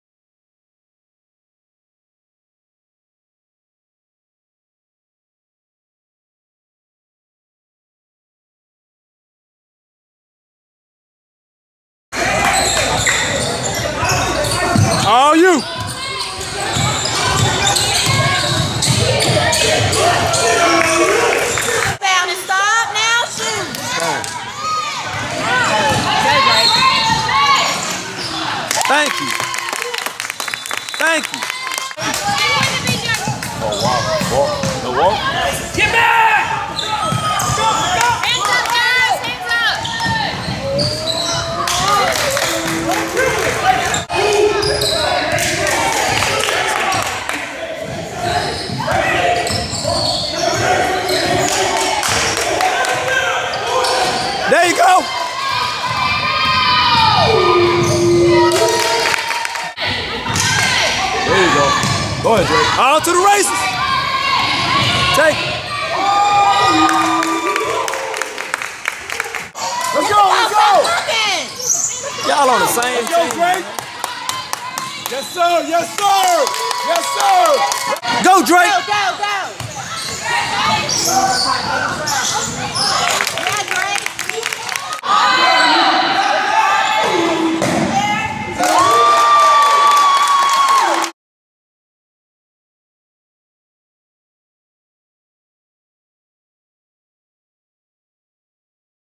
Drake Powell 2017 BCT highlights pt 1 1080p
2024 Drake Powell of Bull City Thunder Elite 2024 (Durham NC) is one of the toughest slashers in his class. This kid is dangerous in transition with great speed, length, agility and defensive prowess. On top of his ability to finish well, he also can knock it down from deep. Has a nose for the ball and is a nightmare for perimeter players.
Drake Powell...Remember I told ya